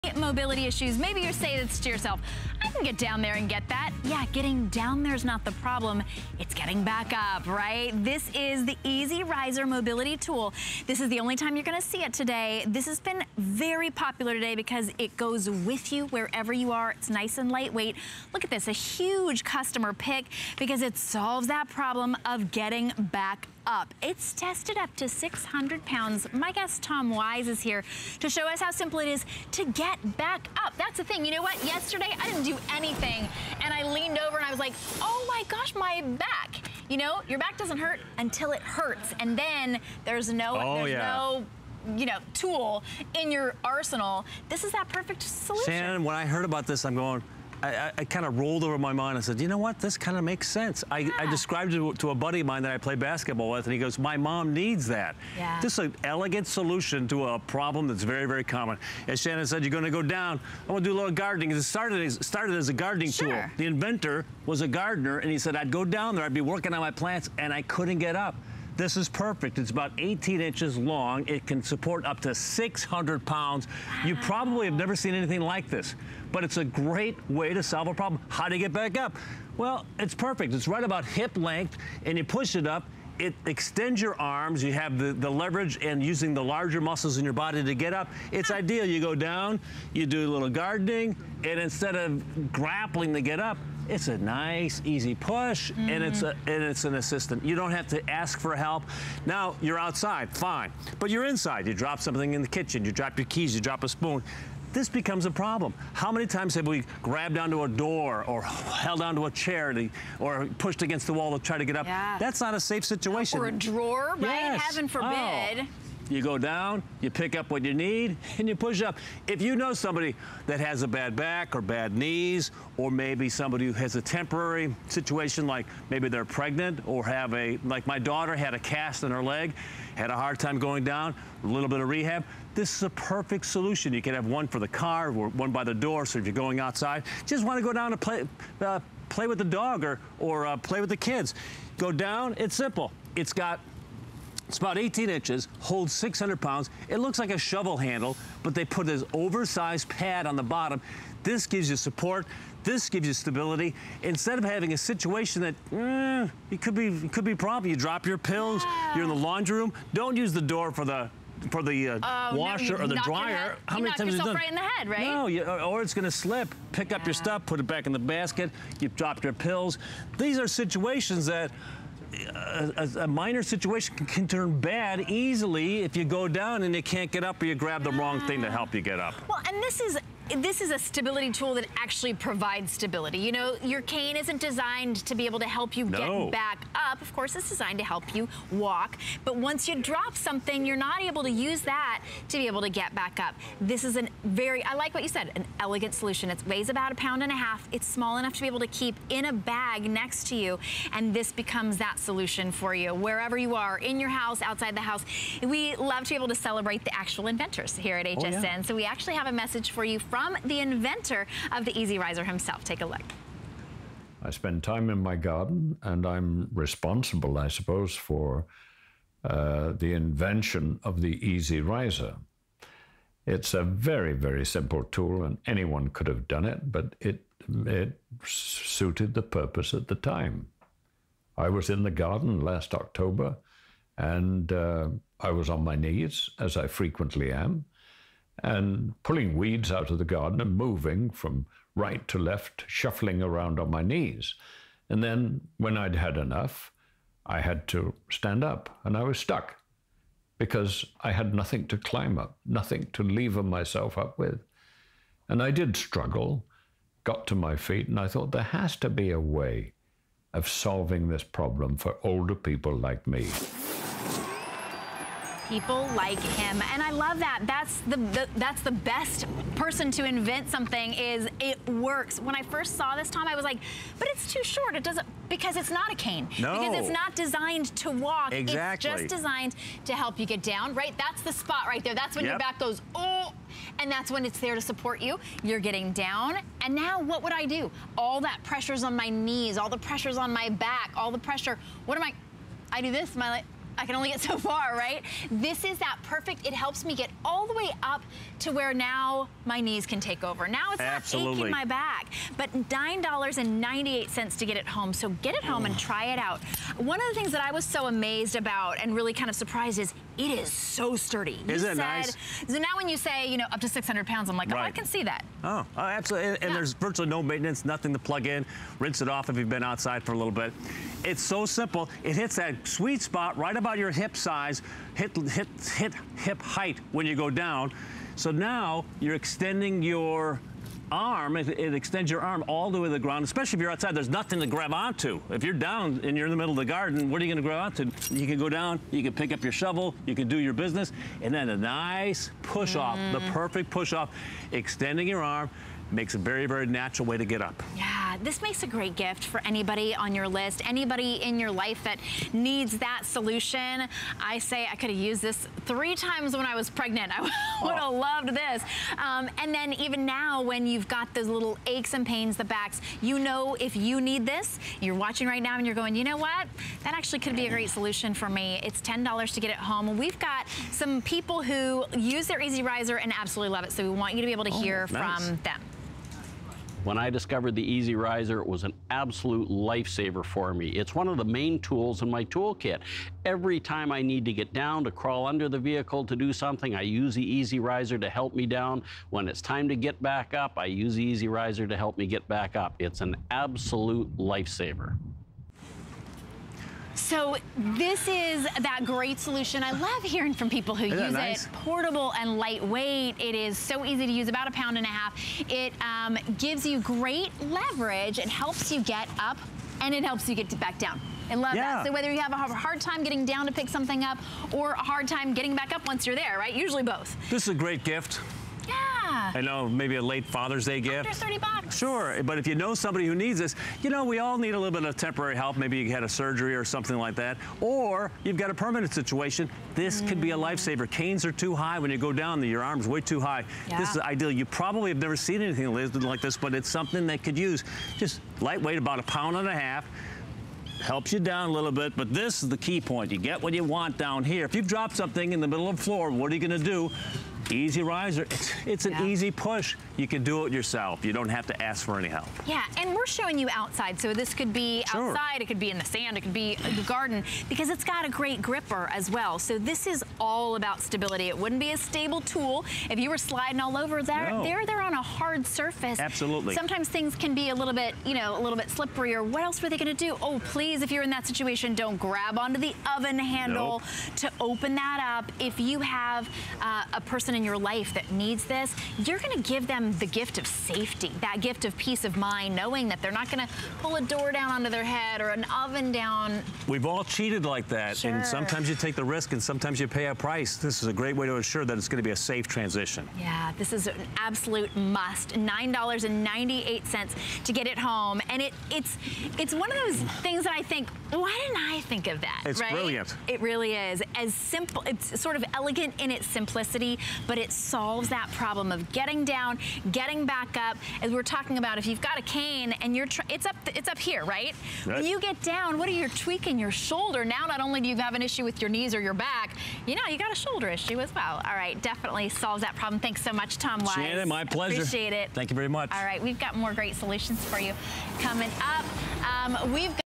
EZRiser Mobility Tool
Everyone knows someone that could use a little extra help getting around ... give them the gift of mobility! The EZRiser makes getting up and down a little easier by working...